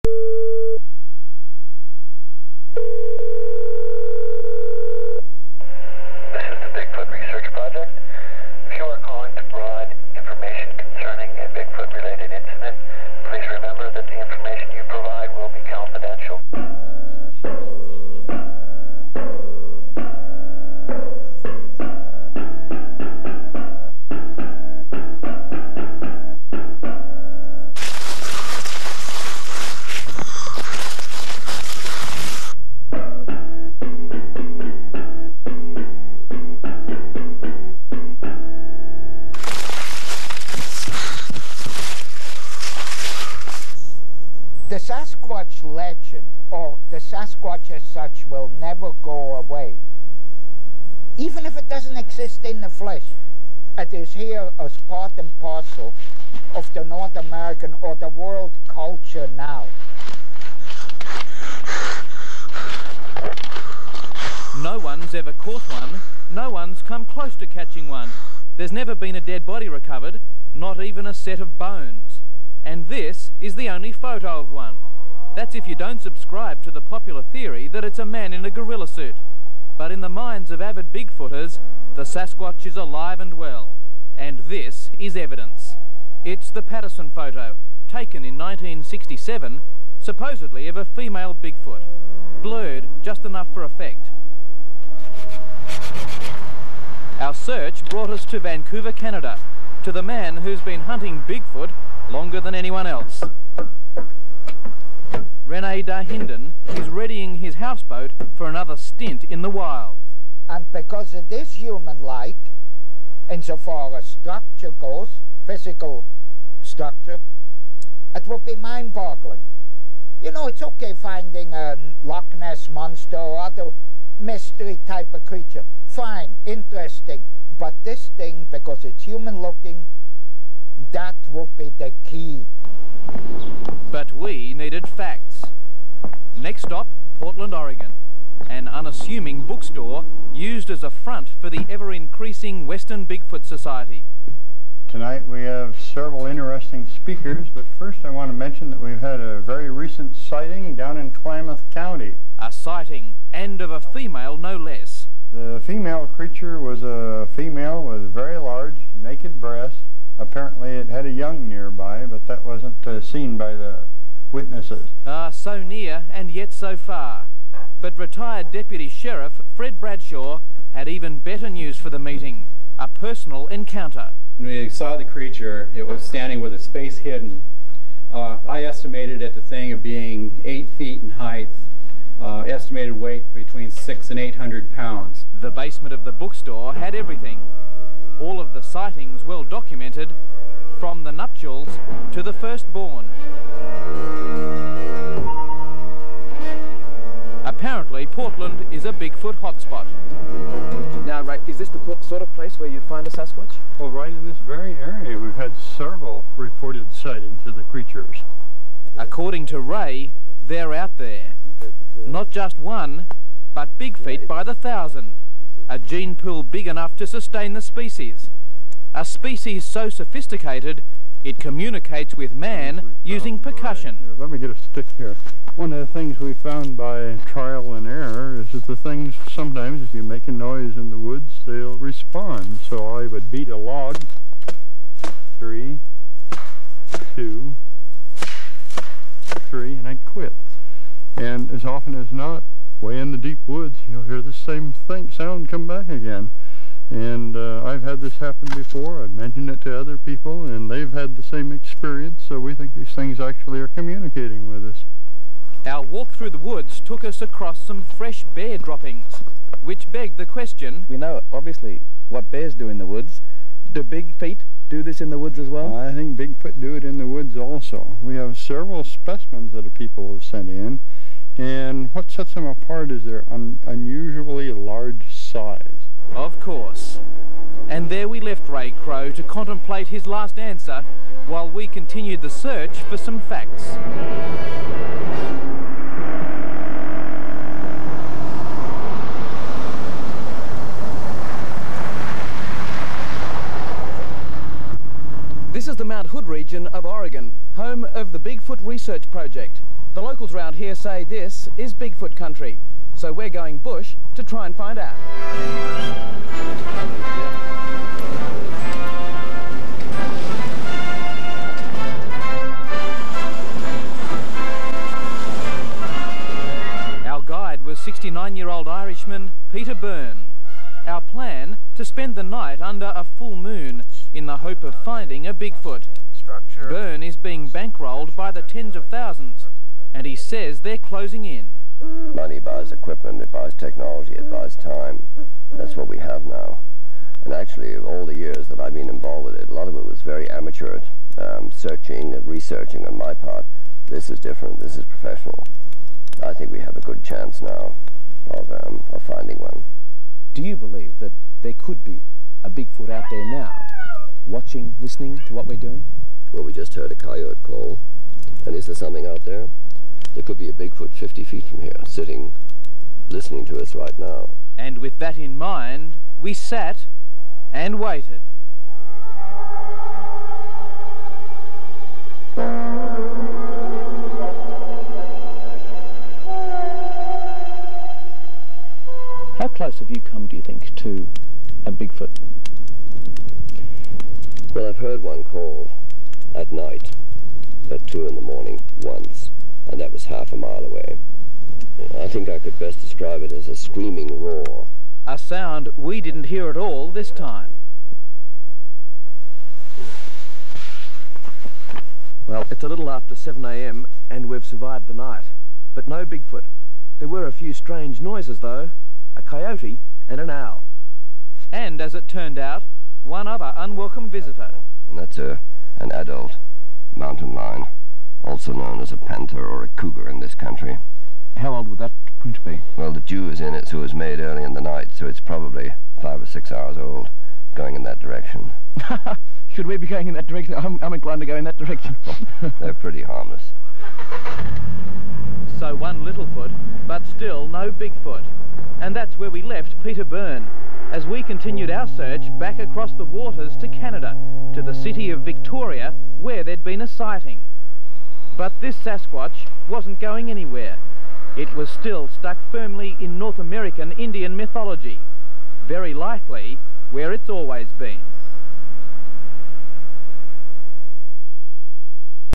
Unknown 1990's Bigfoot Research Project Segment
- Rene Dahinden
- Ray Crowe
- Fred Bradshaw
- "Going Bush" with Peter Byrne near Mount Hood